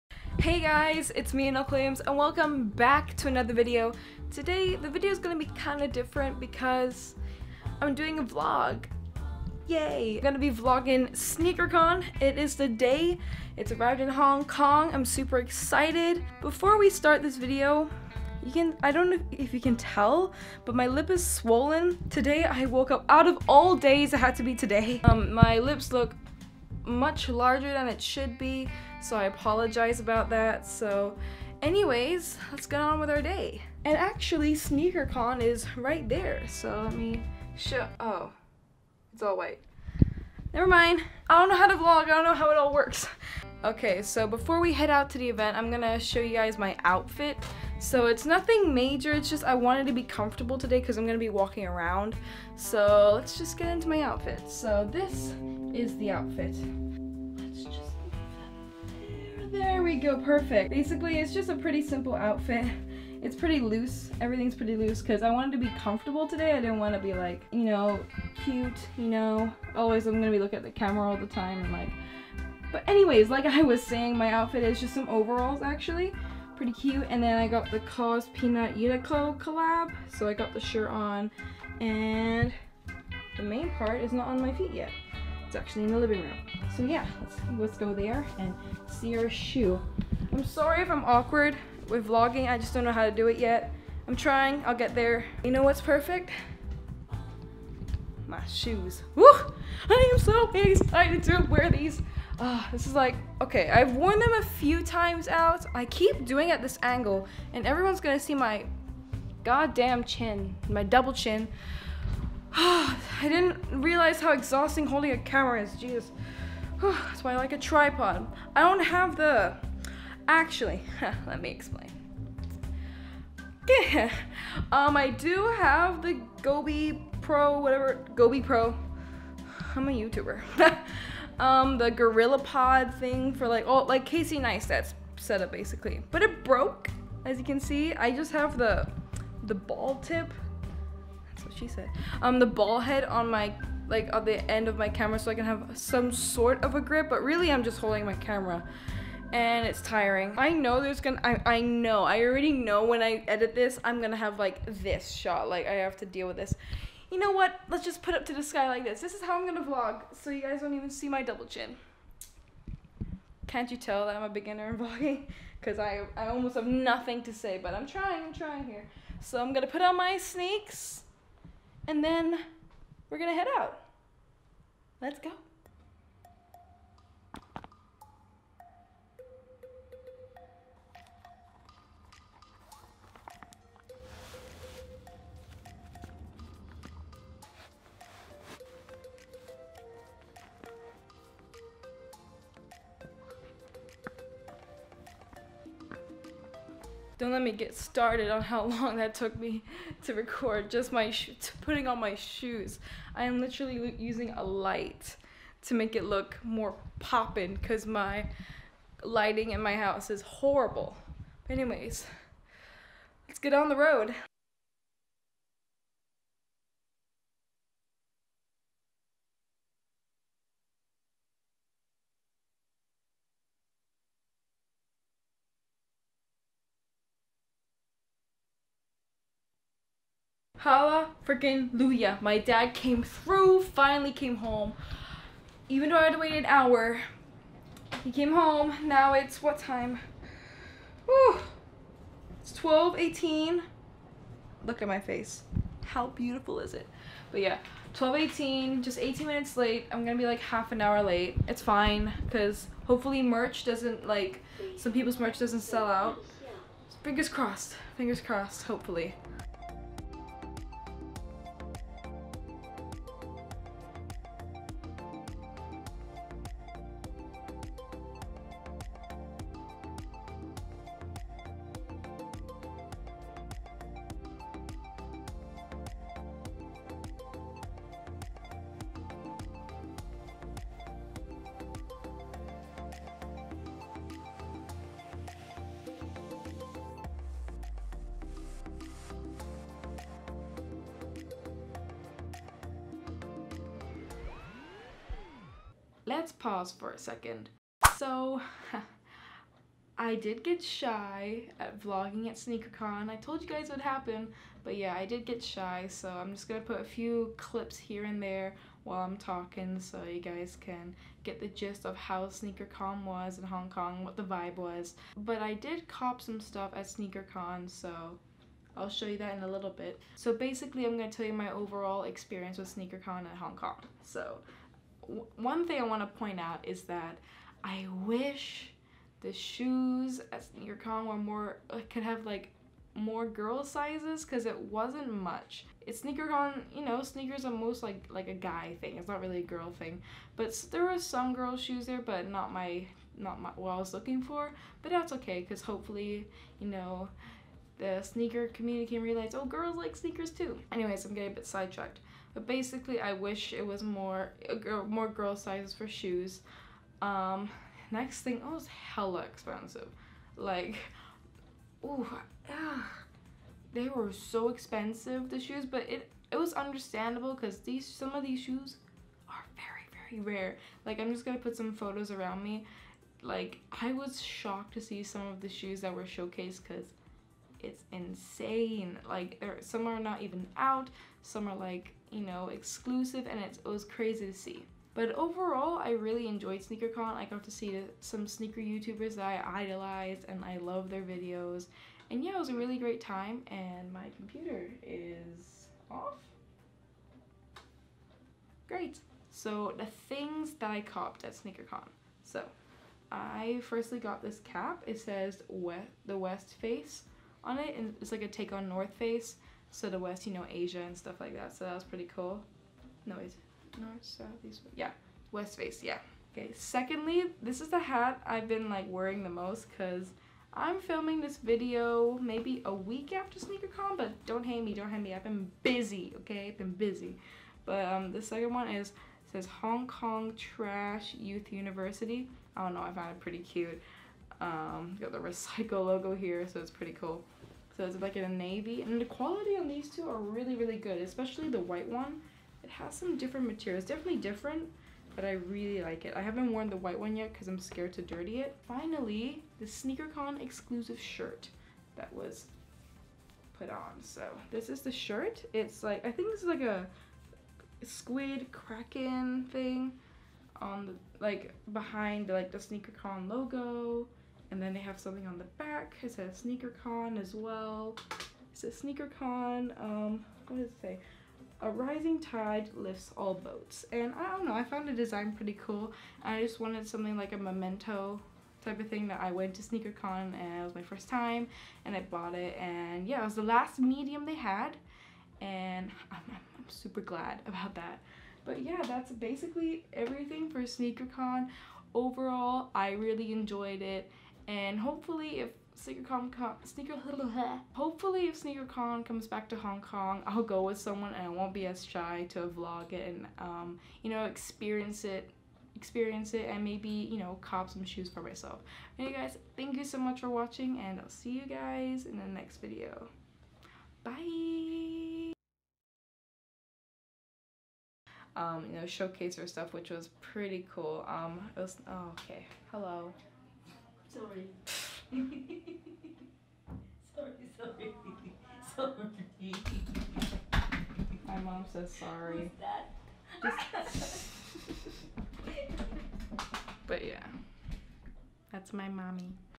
RAREST SNEAKERS IN ASIA! SNEAKERCON HONG KONG 2017 VLOG!
I know the title is clickbait!
Forgive my soul.

dad caps - @dragonmade8
shirt - @sneakercon